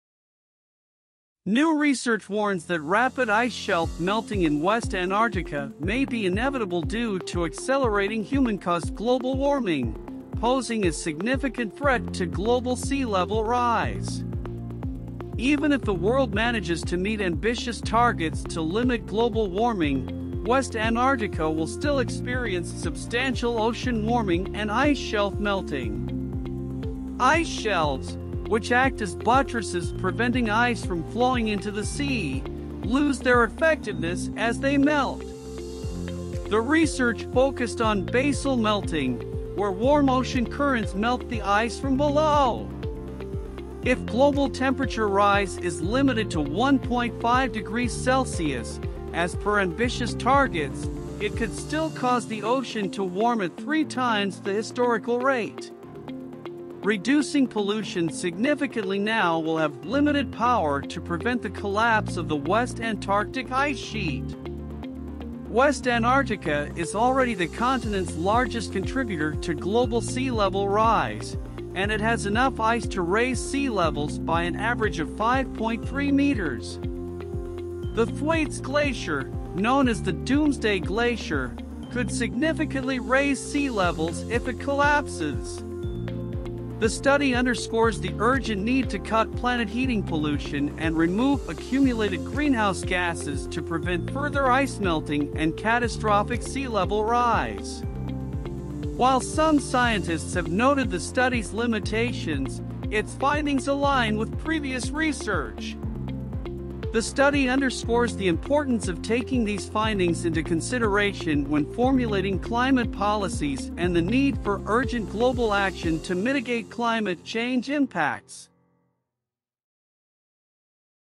Dire Warning Inevitable Melting of West Antarctica's Ice Shelves Threatens Global Sea Levels Made.
New research warns that rapid ice shelf melting in West Antarctica may be inevitable due to accelerating human-caused global warming, posing a significant threat to global sea level rise. Even if the world manages to meet ambitious targets to limit global warming, West Antarctica will still experience substantial ocean warming and ice shelf melting. Ice shelves, which act as buttresses preventing ice from flowing into the sea, lose their effectiveness as they melt. The research focused on "basal melting," where warm ocean currents melt the ice from below.

If global temperature rise is limited to 1.5 degrees Celsius, as per ambitious targets, it could still cause the ocean to warm at three times the historical rate. Reducing pollution significantly now will have "limited power" to prevent the collapse of the West Antarctic ice sheet. West Antarctica is already the continent's largest contributor to global sea level rise, and it has enough ice to raise sea levels by an average of 5.3 meters. The Thwaites Glacier, known as the "Doomsday glacier," could significantly raise sea levels if it collapses.

The study underscores the urgent need to cut planet-heating pollution and remove accumulated greenhouse gases to prevent further ice melting and catastrophic sea-level rise. While some scientists have noted the study's limitations, its findings align with previous research. The study underscores the importance of taking these findings into consideration when formulating climate policies and the need for urgent global action to mitigate climate change impacts.